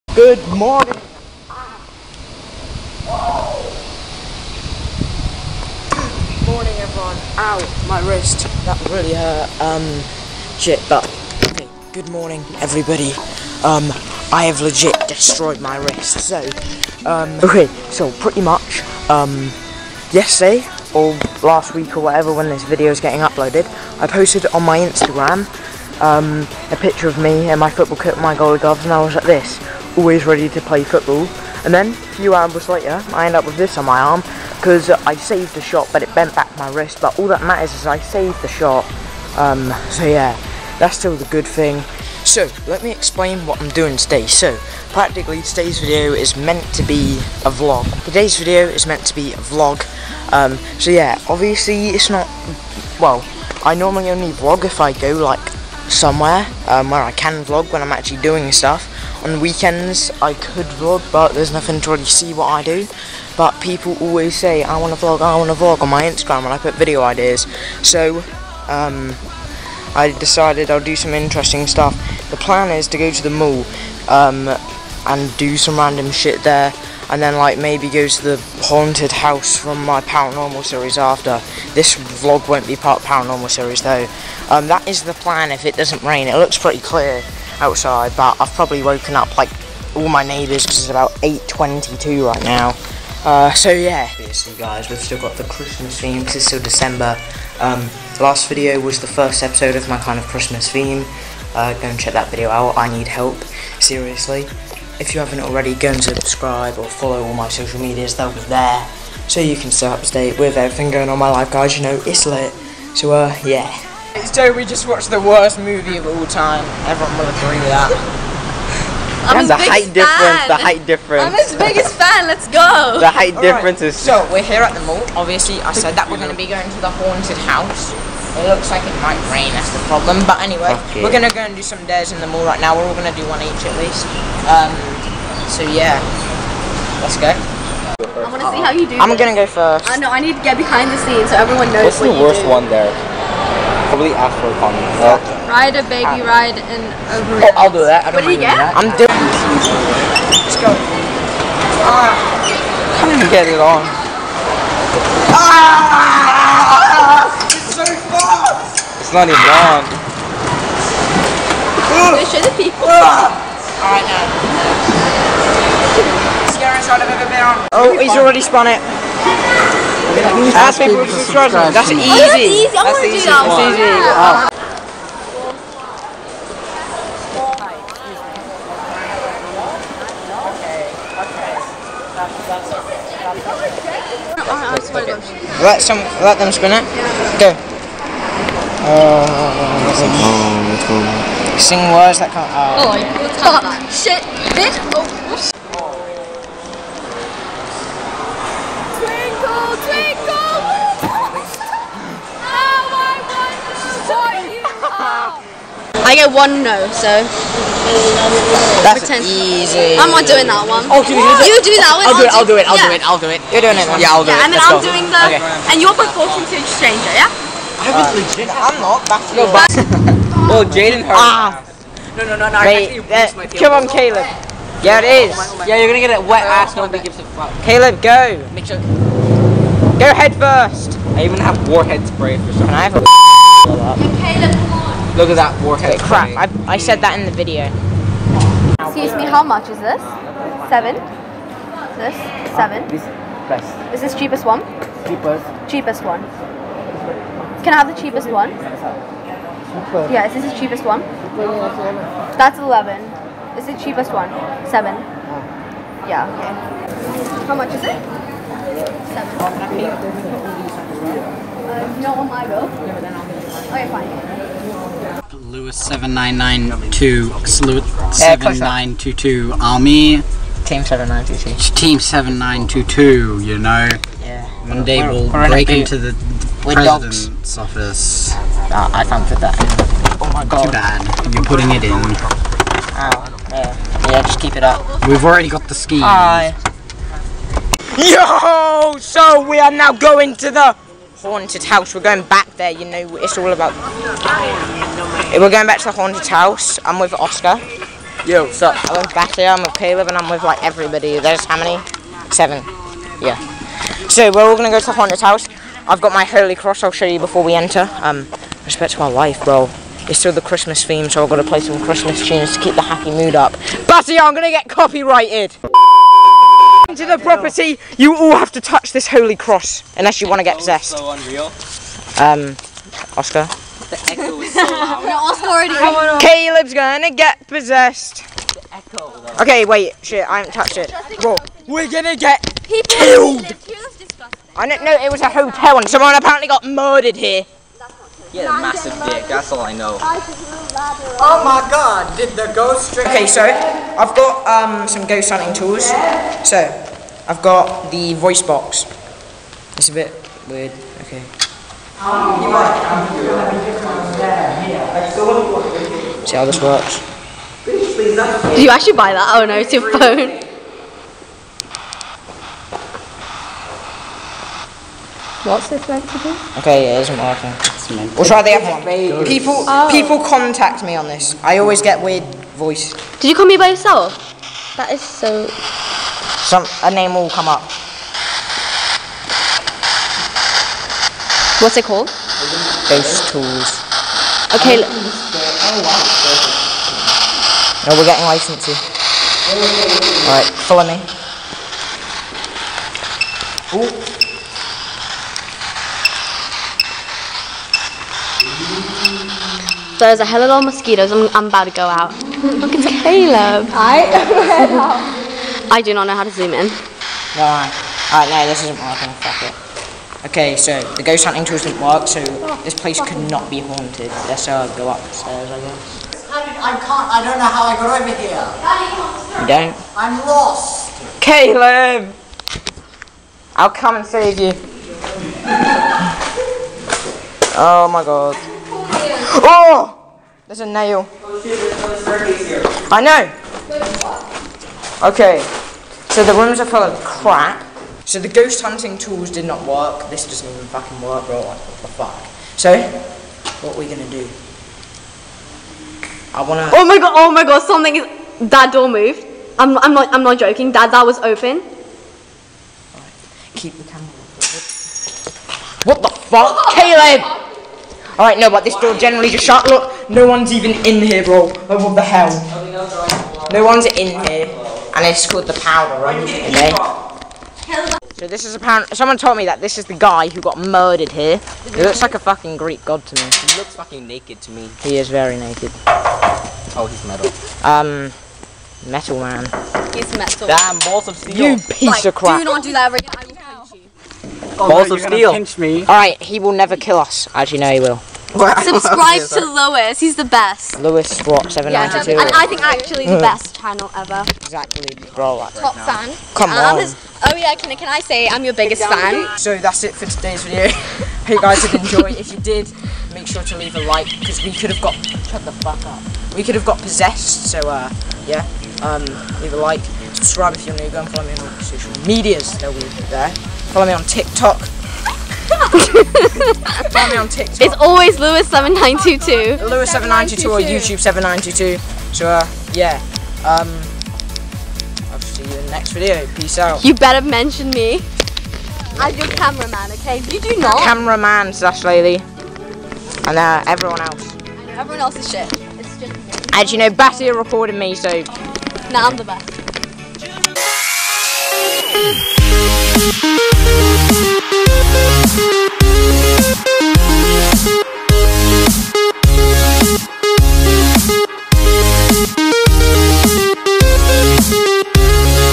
I WENT BACK TO THE HAUNTED MANSION
I went back to the haunted mansion, I also decided to do a few other things for the video and put it in a vlog because you all want a vlog.